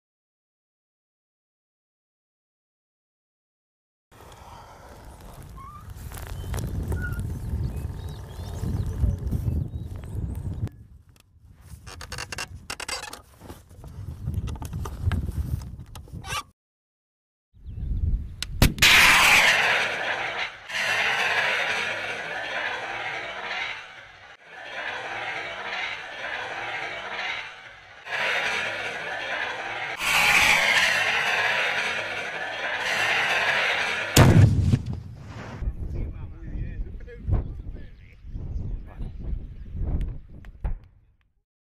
Javelin Missile Training
NATO allies conduct Javelin missile training on a decommissioned tank.